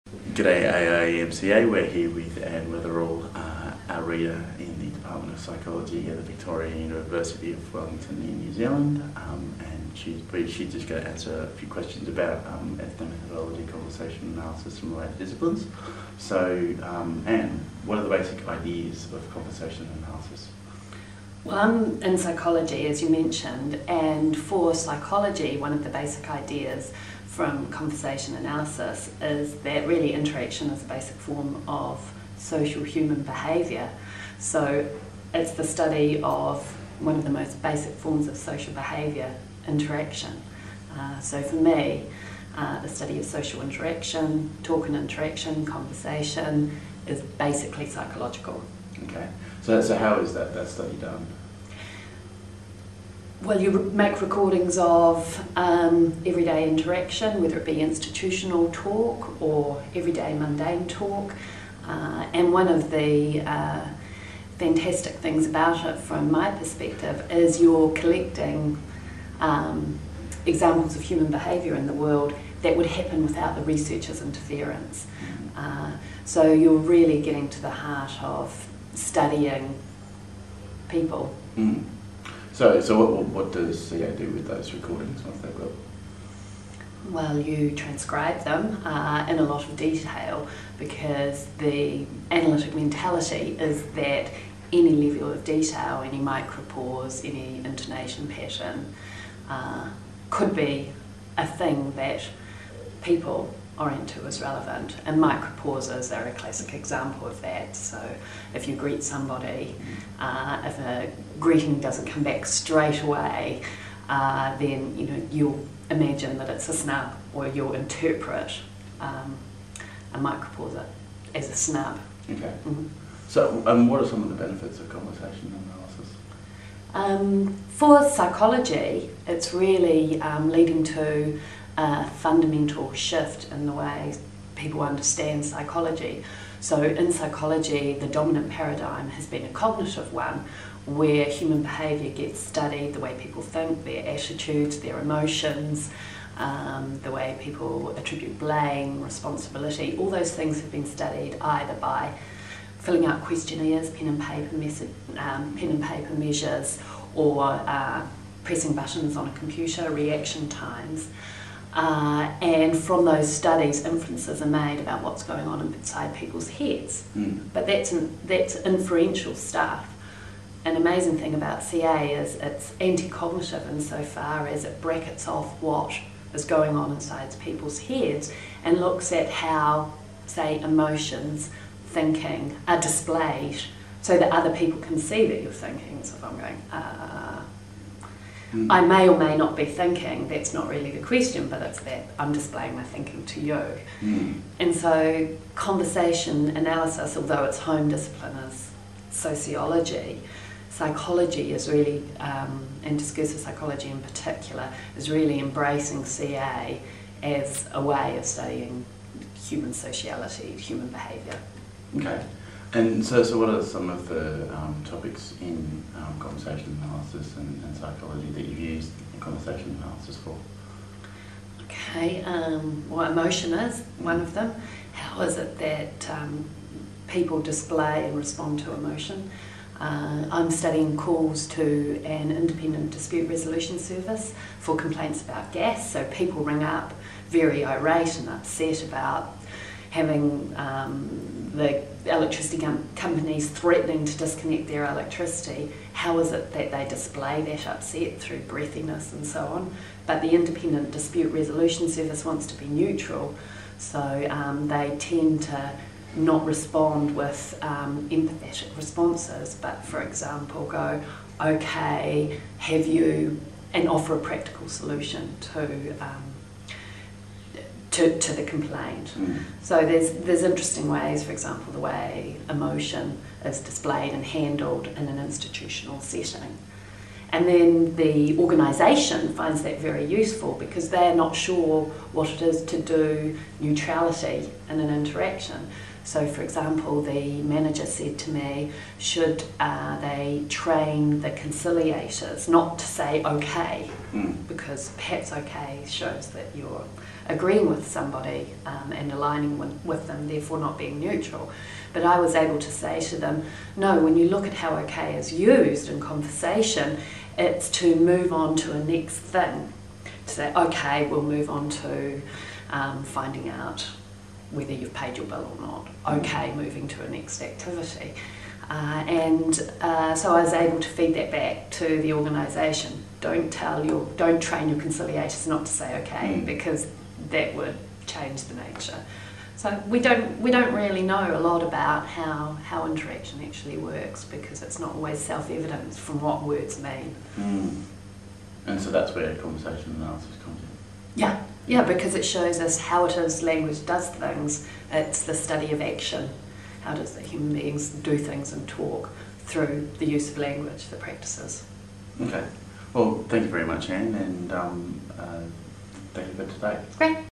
Ann Weatherall: IPRA Diaries 2009
Ann Weatherall discussing conversation analysis and it's usefulness in Psychology